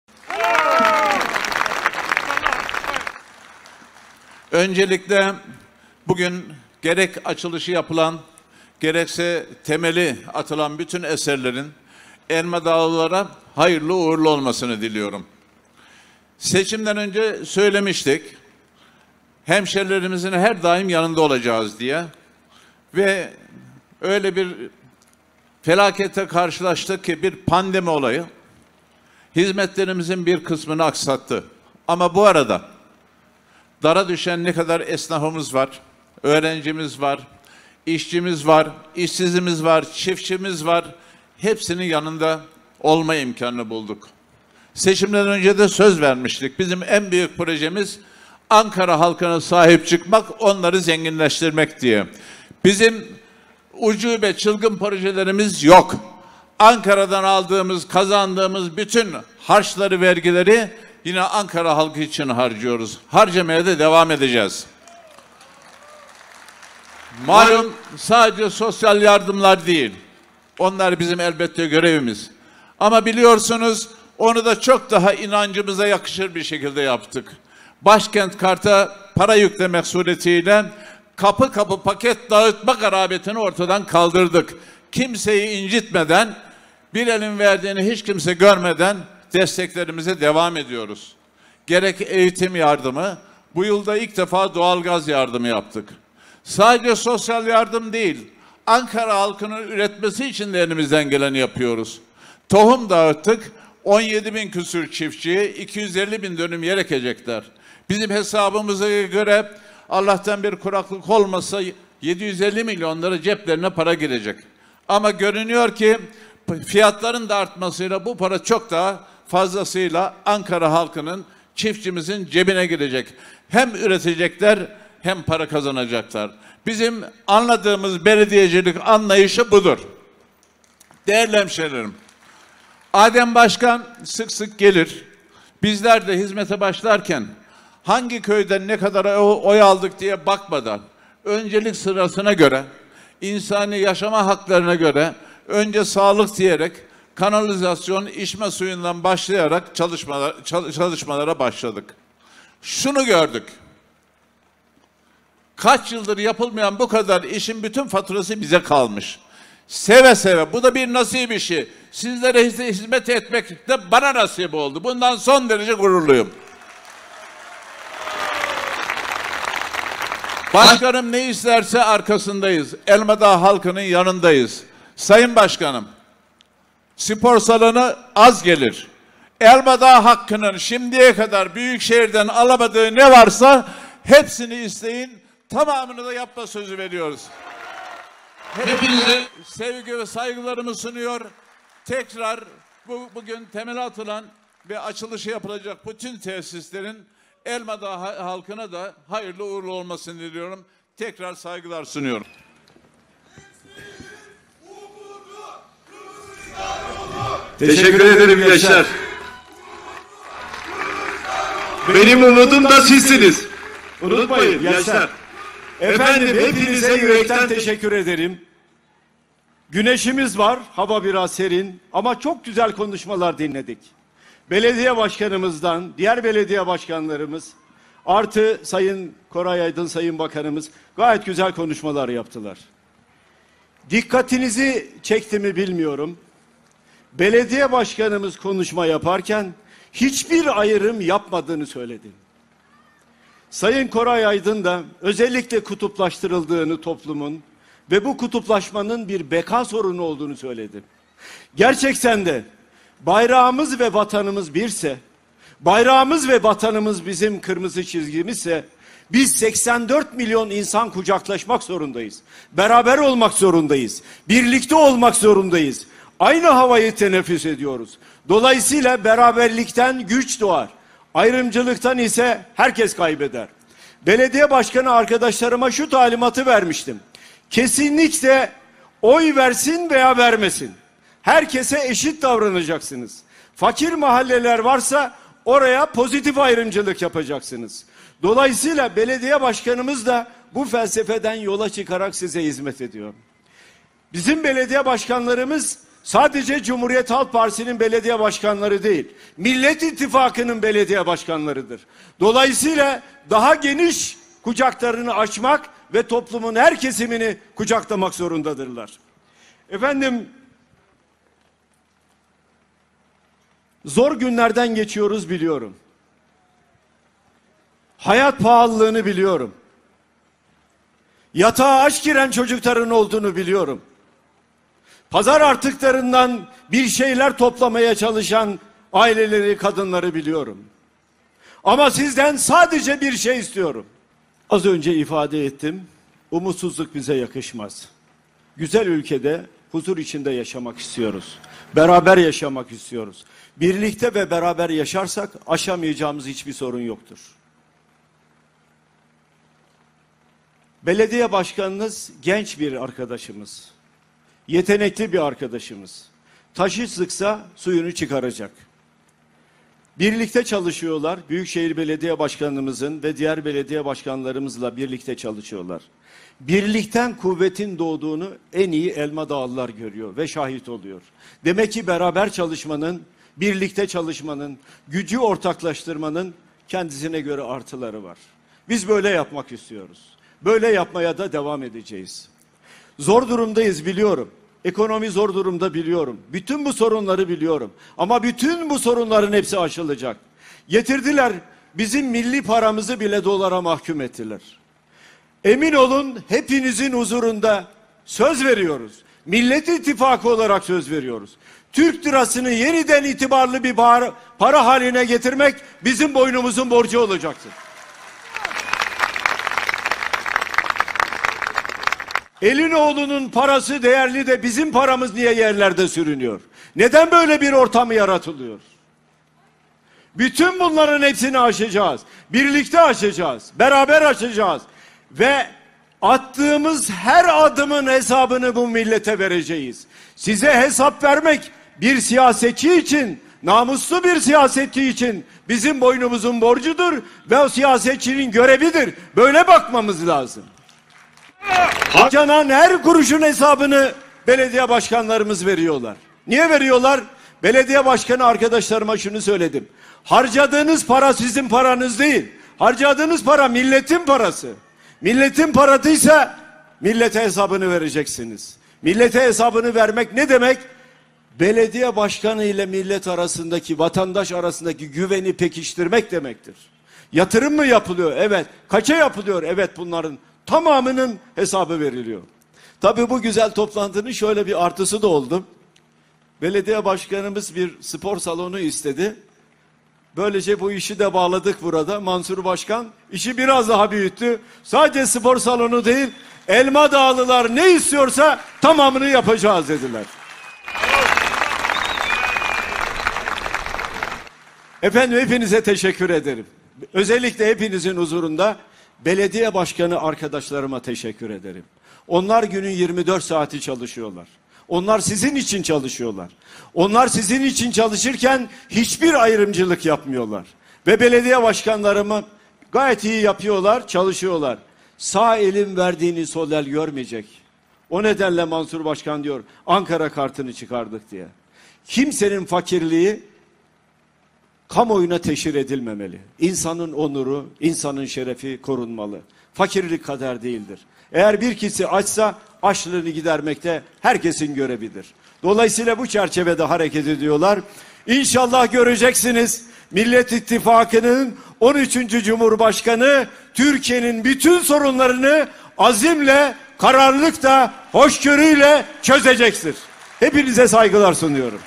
Kemal Kılıçdaroğlu Mansur Yavaş ile bir arada! Çarpıcı Cumhurbaşkanlığı açıklaması!
CHP Genel Başkanı Kemal Kılıçdaroğlu Ankara Büyükşehir belediye başkanı Mansur yavaş ile bir araya geldi ! Konuşmalarında iktidara yüklenen CHP'li Başkanlardan Mansur Yavaş'ın " Bizim ucube,çılgın projelerimiz yok!" sözleri büyük alkış topladı !